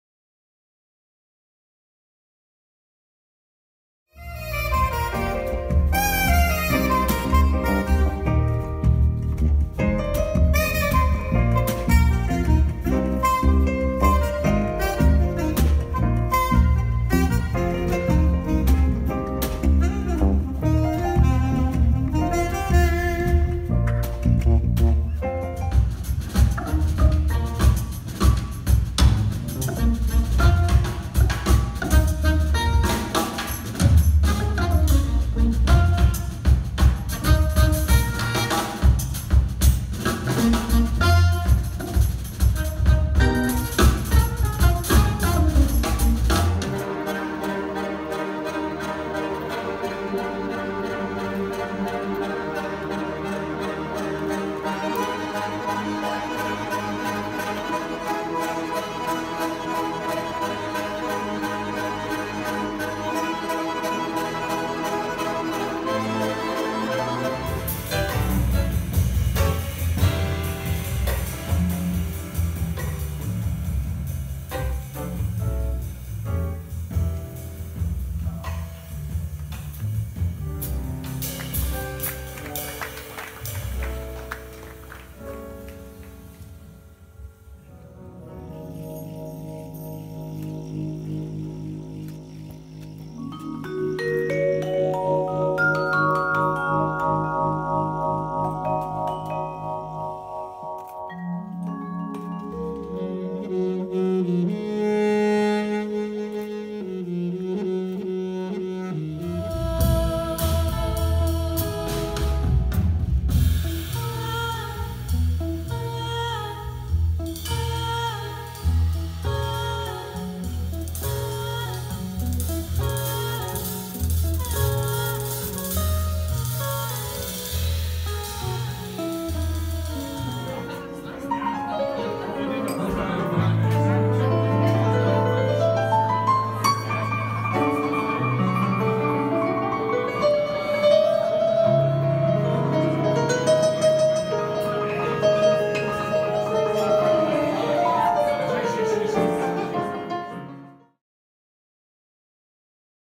Jazz & Wine of Peace 2016
Concerts highlights from Jazz & Wine of Peace 2016 jazz festival - Cormons, Italy.